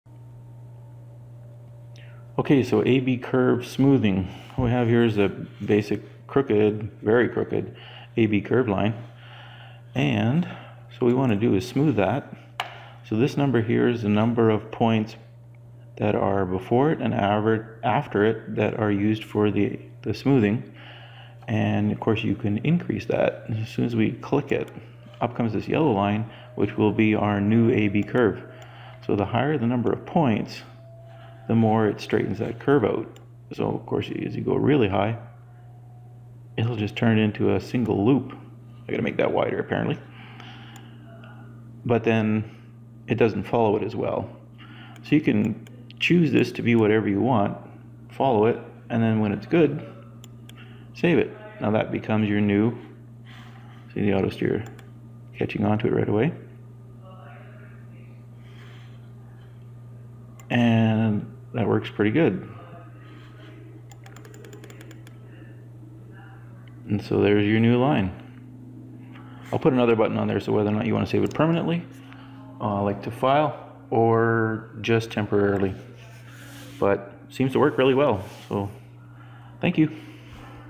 AB Curve Smoothing
Smoothing the ripples and bumps out of a driven AB Curve. Center Point Averaging method